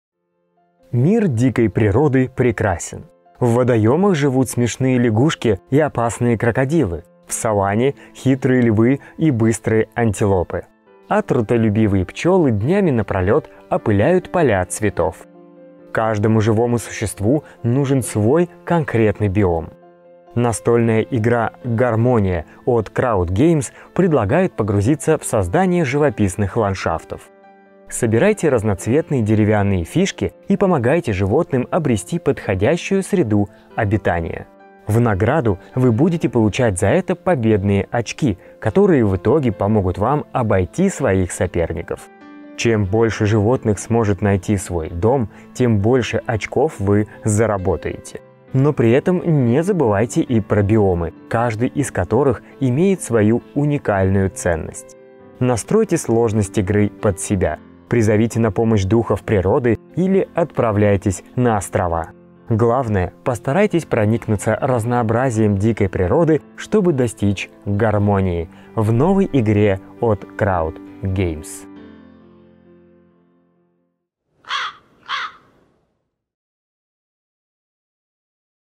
Настольная игра «Гармония» — краткий обзор
Приятная семейная игра про создание идеального крошечного мира, в котором животные существуют в гармонии с природой!

В настольной игре «Гармония» вы будете возводить на своих планшетах ареалы, используя цветные фишки ландшафтов 6 типов, и заселять их различными животными.

Это одна из тех обманчиво простых стратегических игр, в которых крайне важно каждое ваше решение! Ведь для того, чтобы одержать победу, вам будет нужно принимать исключительно грамотные решения при размещении фишек ландшафтов. Изюминки и глубины добавляет ещё и тот факт, что задача, которая предстаёт перед игроками, стоит не только в горизонтальной плоскости, но и в вертикальной!